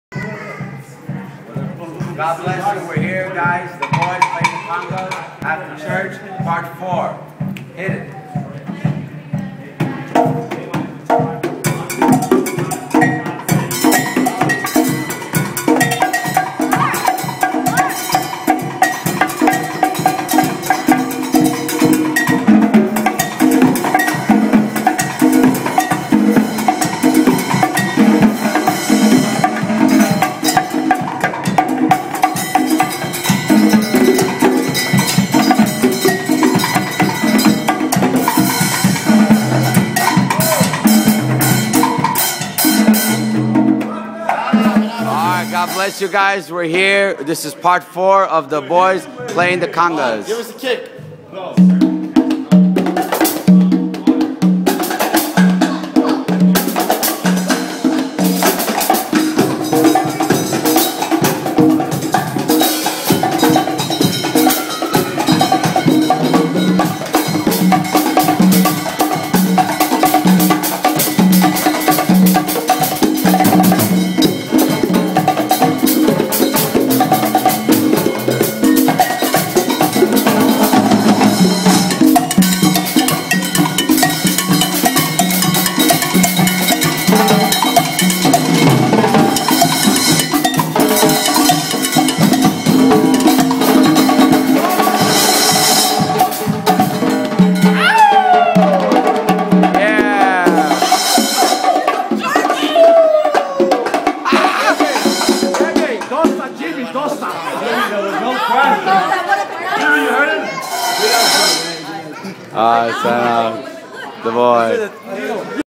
Gods Gypsy Christian Church Queens NY Boys Playing Bongos After Church Part 4
After church at pastor Lavie church queens NY GGCC